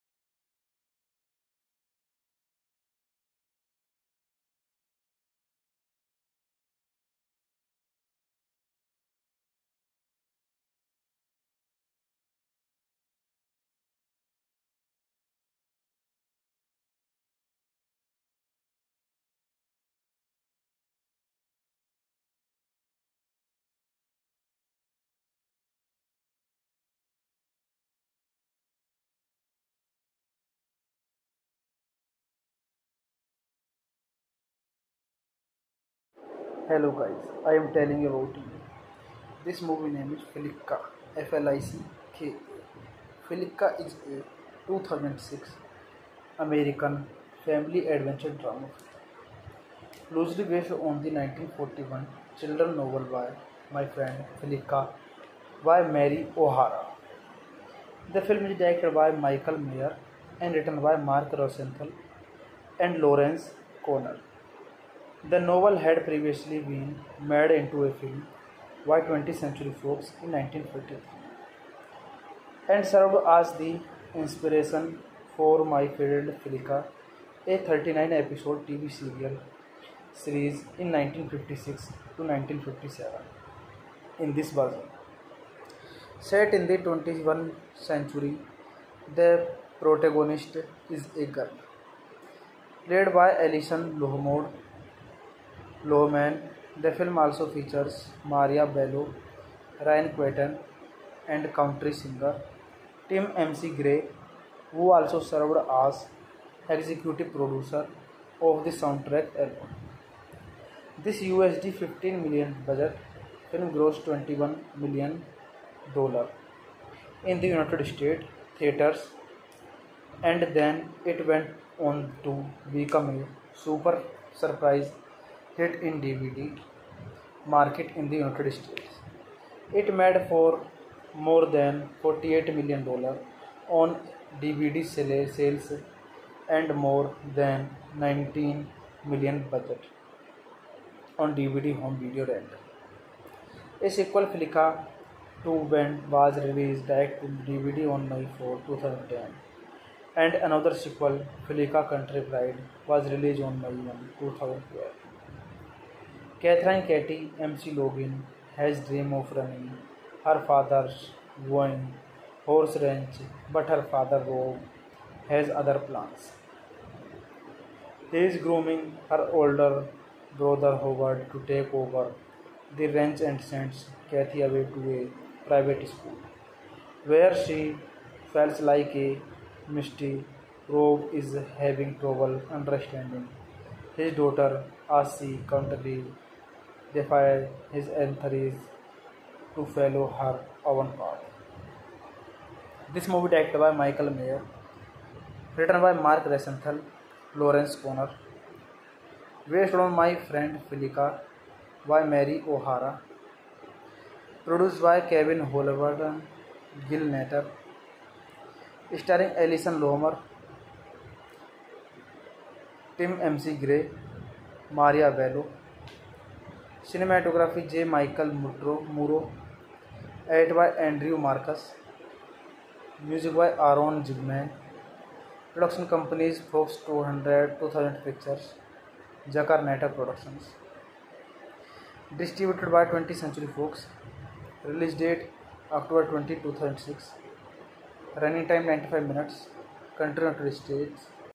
Flicka Full Movie Facts And REview In English / Alison Lohman / Tim McGraw
In this video we will tell you about fact and review of movie Flicka .

Note: This video is for fact knowledge and entertainment purposes only.